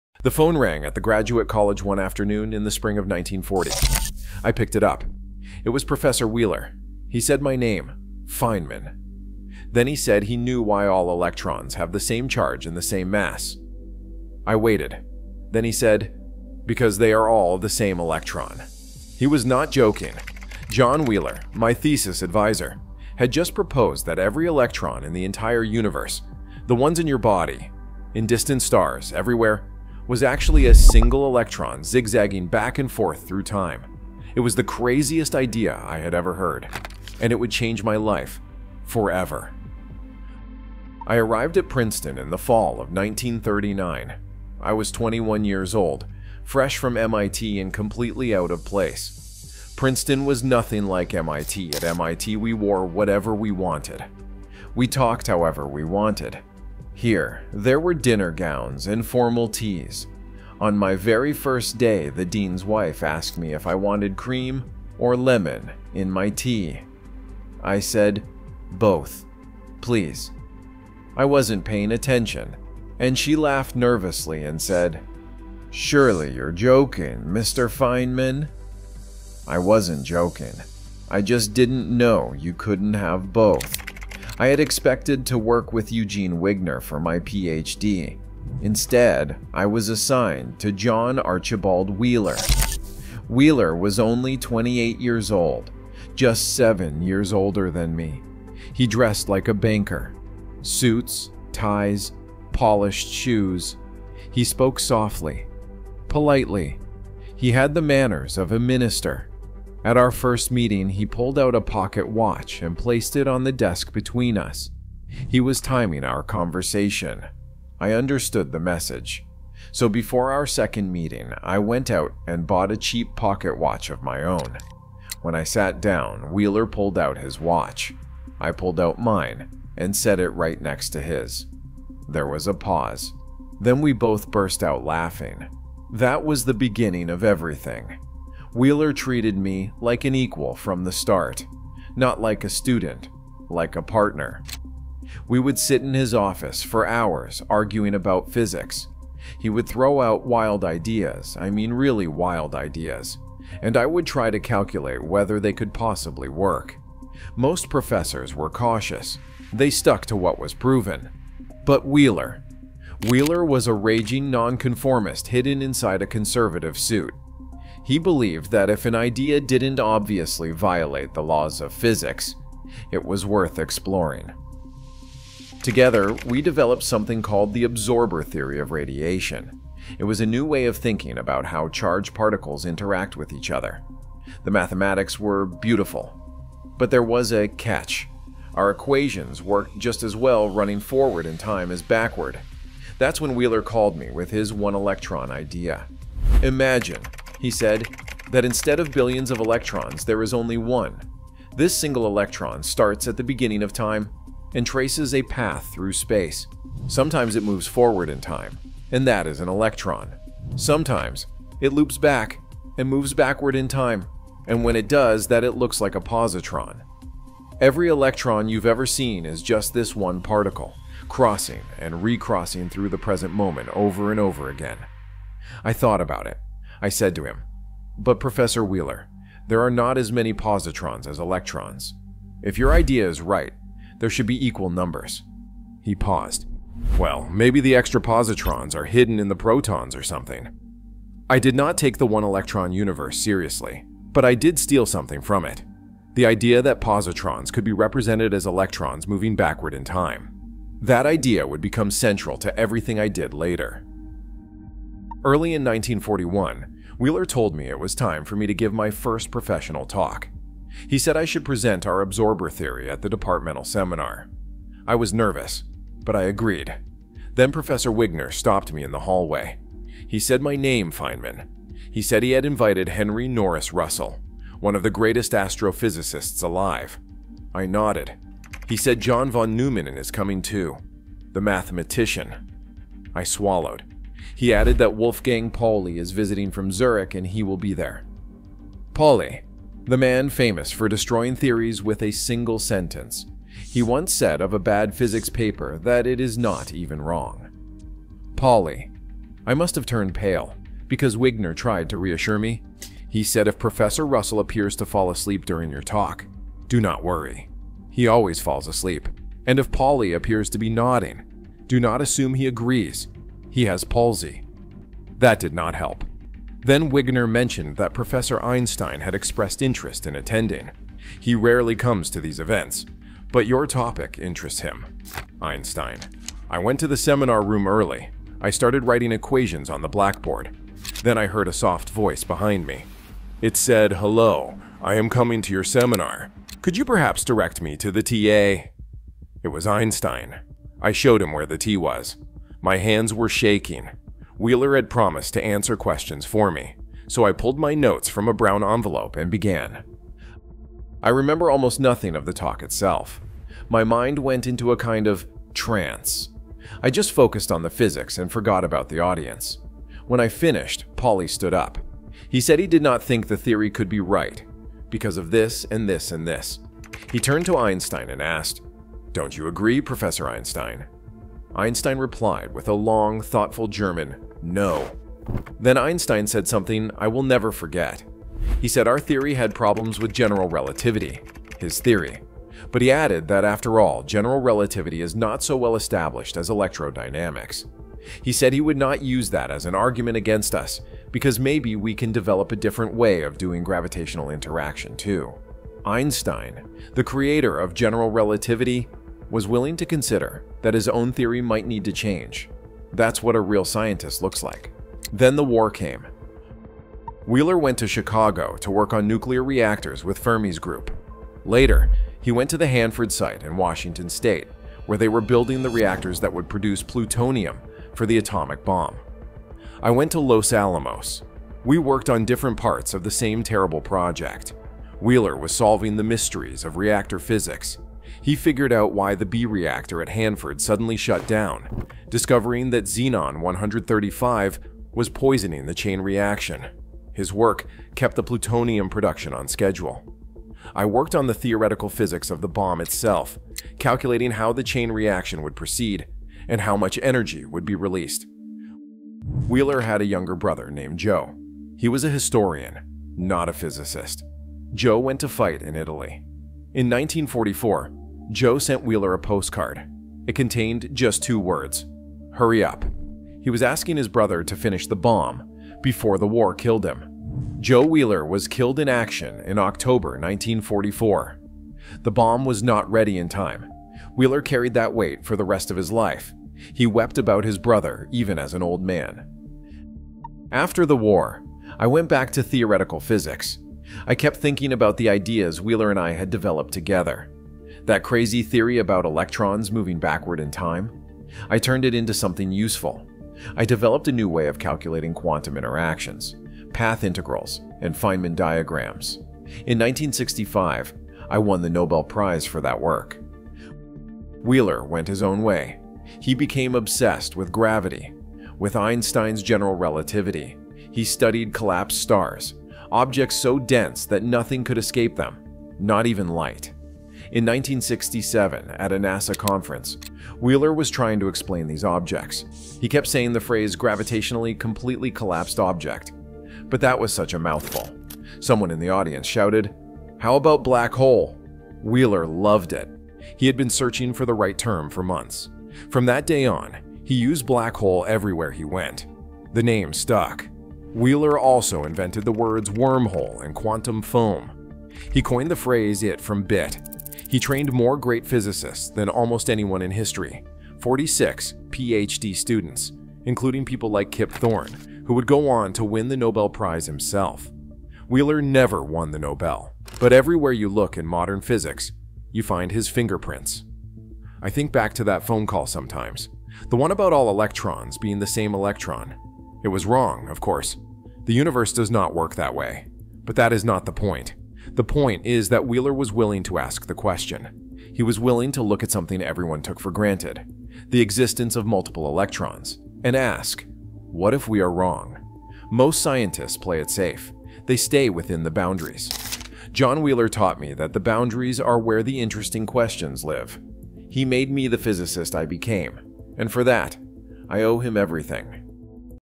Richard Feynman Learned From The Man Who Named BLACK HOLES
The phone rang at the graduate college one afternoon in the spring of 1940. It was Professor Wheeler. "Feynman," he said, "I know why all electrons have the same charge and the same mass. Because they are all the same electron." It was the craziest idea I had ever heard. And it would change my life forever.

This is the story of how John Archibald Wheeler, the man who named black holes, became my mentor, my collaborator, and the person who shaped everything I would become.

This video is a dramatized retelling of real historical events, narrated from Richard Feynman's perspective. While based on documented accounts, some dialogue has been recreated for narrative purposes. This is not an official transcript.

CHAPTERS
0:00 - The Phone Call
0:40 - Arriving at Princeton
1:23 - Meeting Wheeler
2:18 - A Partnership of Equals
3:12 - The One-Electron Universe
4:14 - My First Seminar
5:28 - Einstein Speaks
6:46 - The War
7:31 - "Hurry Up"
8:02 - After the War
8:38 - Black Holes
9:25 - What Wheeler Taught Me

📖 SOURCES
• "The Quantum Labyrinth: How Richard Feynman and John Wheeler Revolutionized Time and Reality" - Paul Halpern
• "Surely You're Joking, Mr. Feynman!" - Richard Feynman
• "Genius: The Life and Science of Richard Feynman" - James Gleick
• "Geons, Black Holes, and Quantum Foam: A Life in Physics" - John Archibald Wheeler
• Feynman's Nobel Lecture (1965)
• Princeton University Physics Department Archives